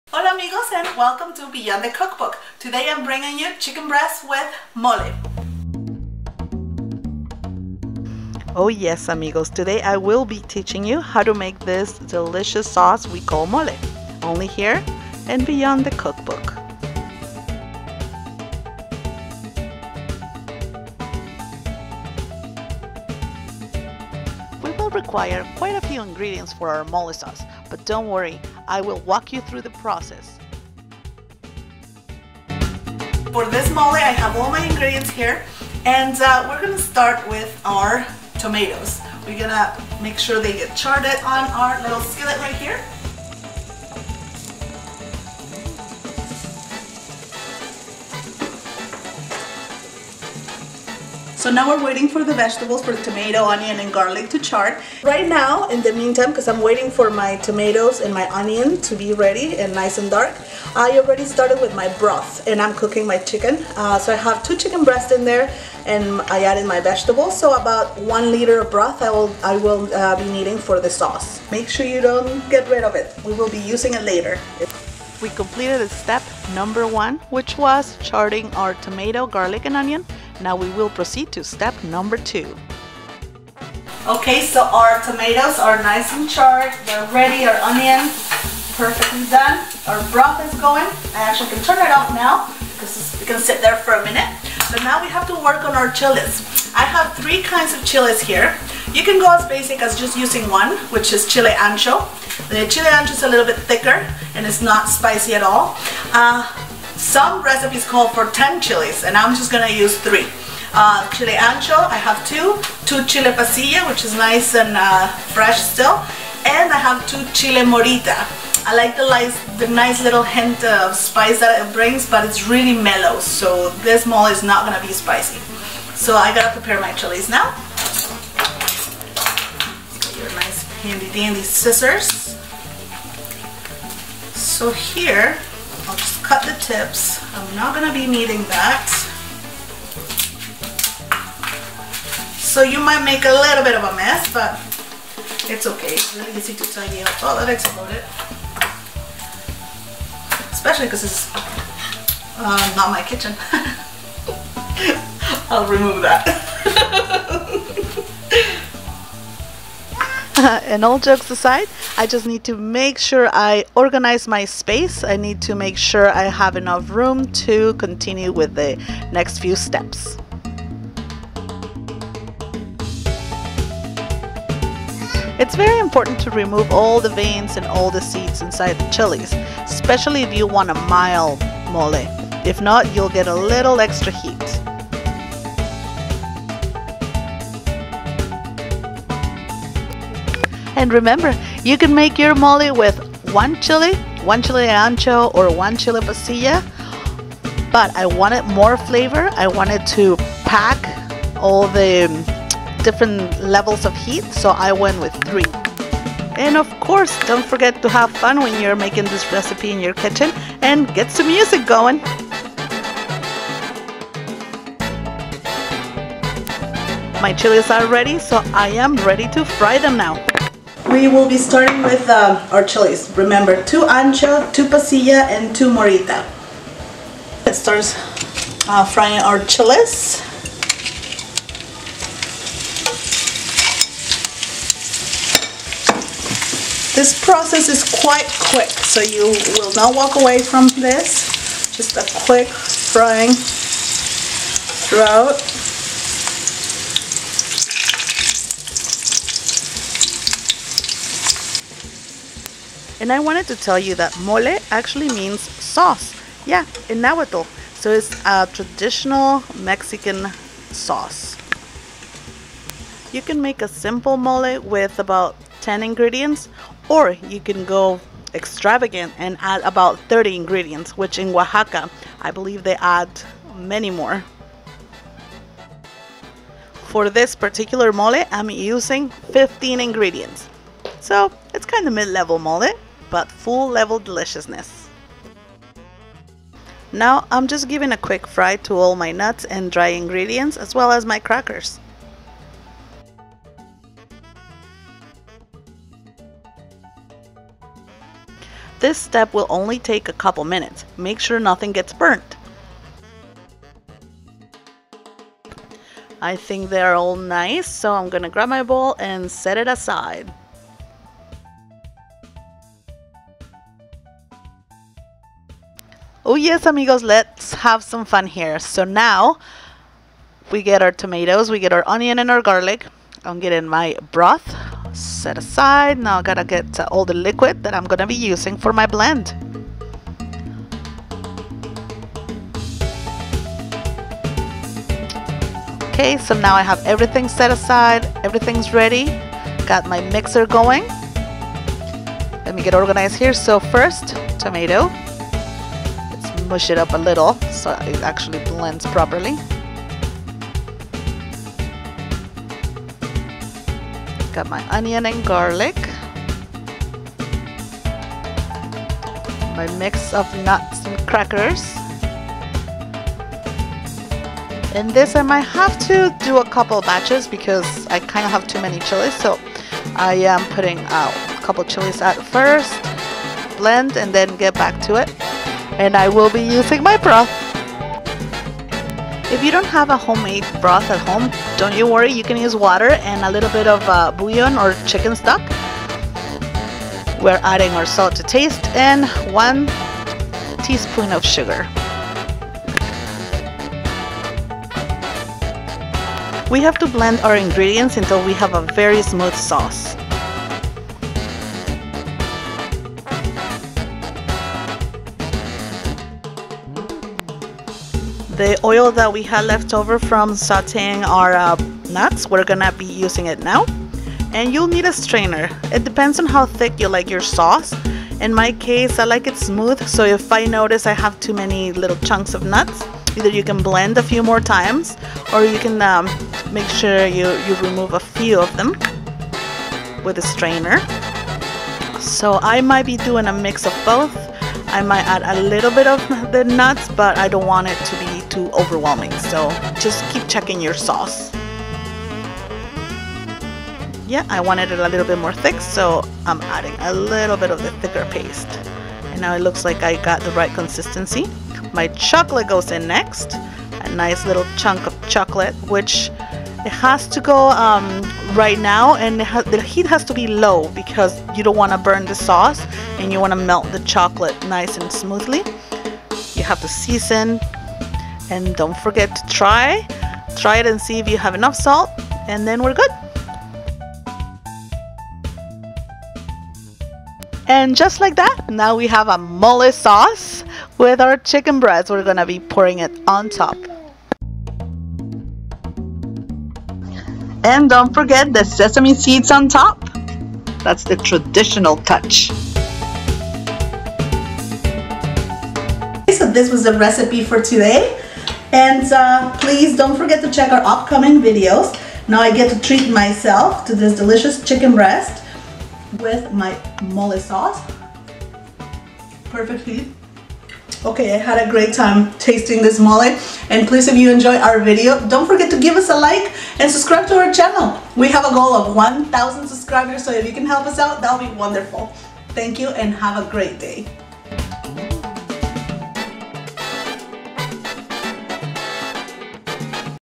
Chicken Breast With Traditional Mexican Mole Sauce
I will show you how to make our Mexican Mole Sauce, it goes perfect with chicken, You have to try it!